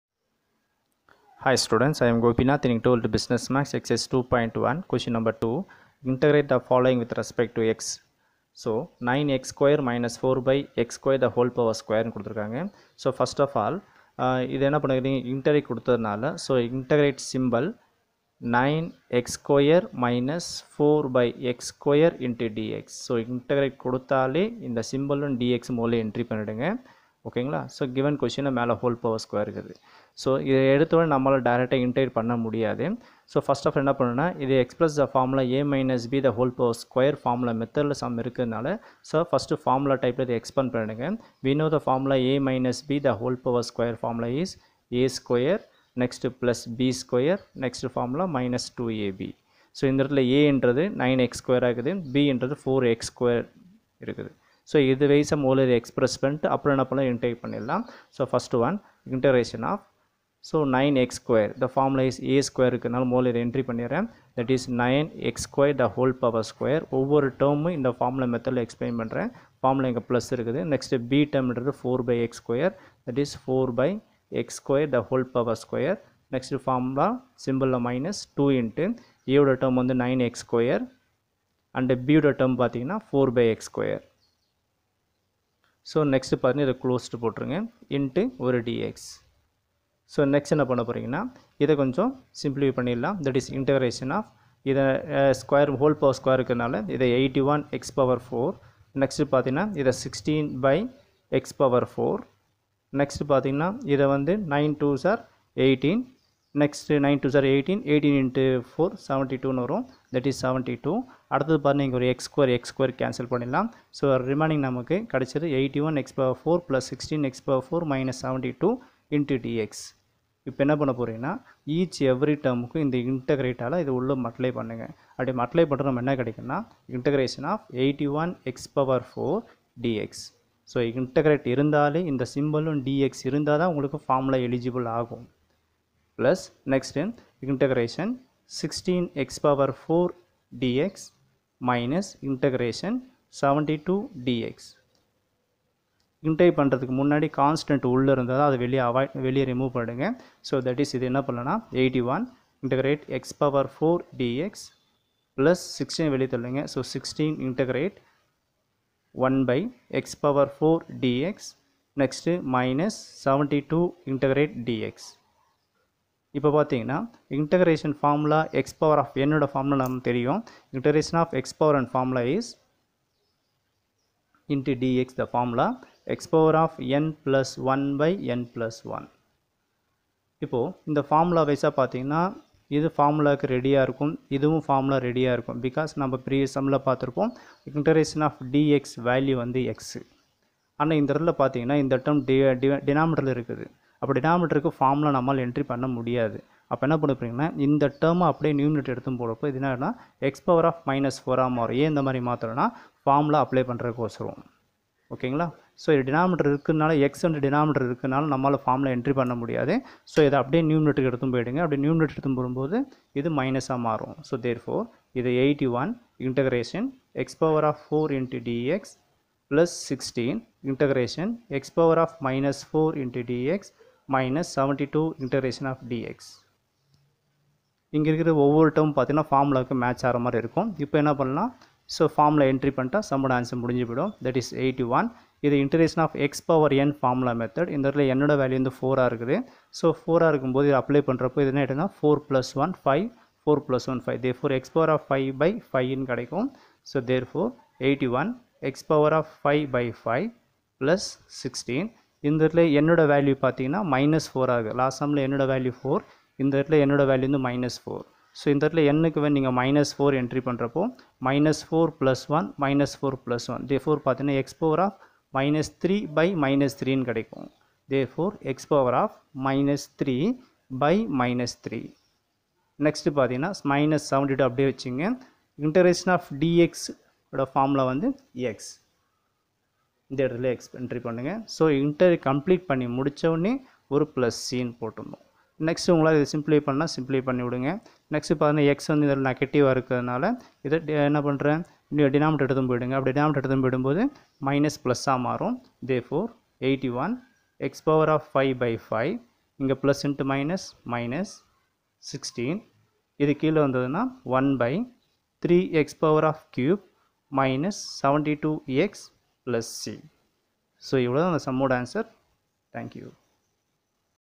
TN 12th Business maths chapter 2 integral calculus exercise 2 .1 q . no 2
TN New syllabus class 12 bussiness maths
Chapter 2 integral calculus exercise 2.1 question number 2